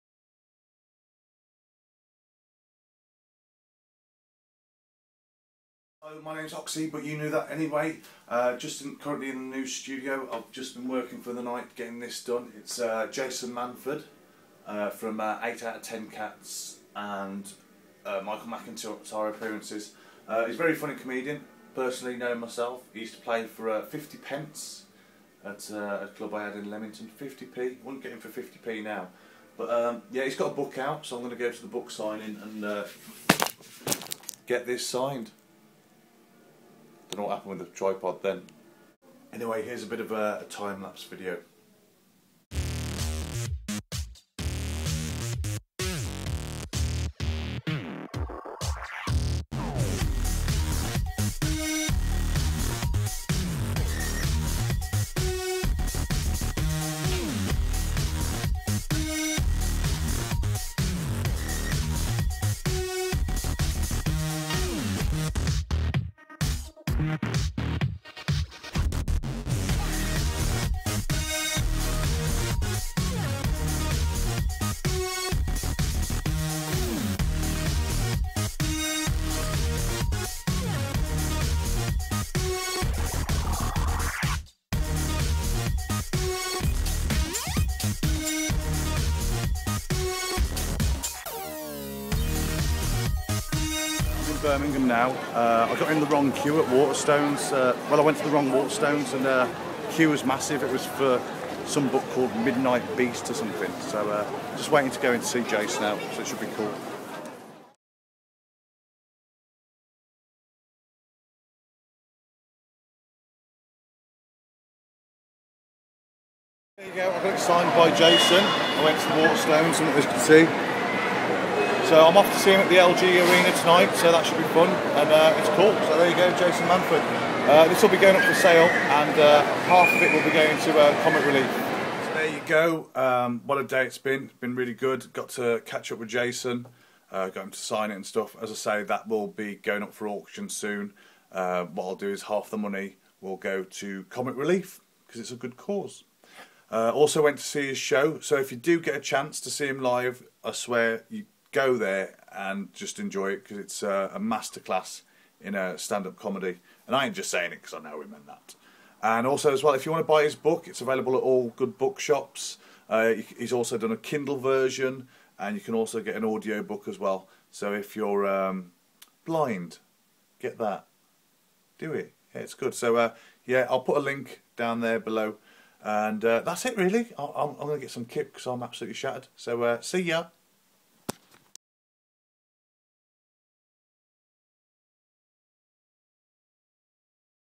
Okse Paints Jason Manford
I went to go see Jason Manford and asked him to sign a painting i did of him. Which he kindly did.

He's currently on your if you get a chance go see him it's a master class in stand up, very slick and funny.

he's also got a book  out now called "Brung Up Proper"

if you'd like to grab a copy its available here

I will be putting up this piece up for auction soon and half the money will go to Comic Relief.

so watch this space.

to be notified when that happens please join my facebook group

cheers

Okse